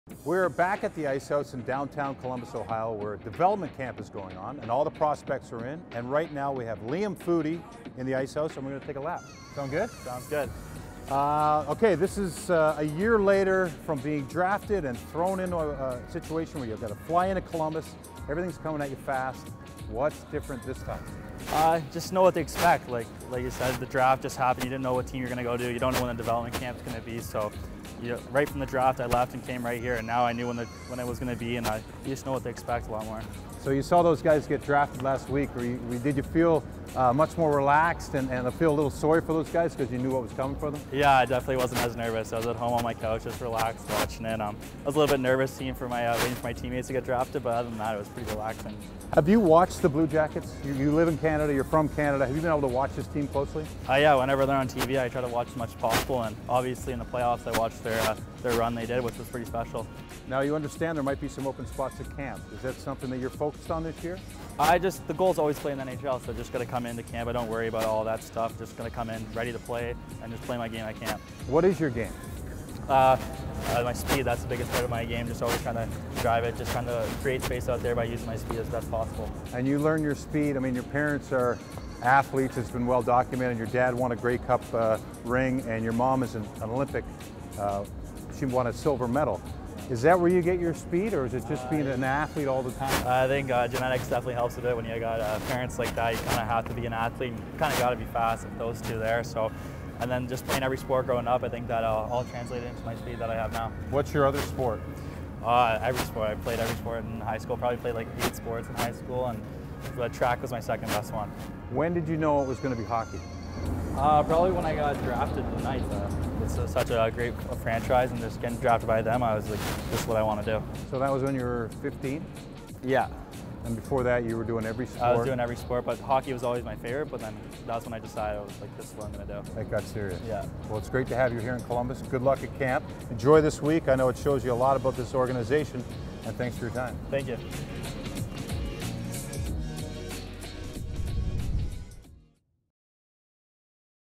One Lap with Jody Shelley: Liam Foudy
Jody is back for another lap at 2019 CBJ Development Camp, this time with 2018 first round draft pick Liam Foudy!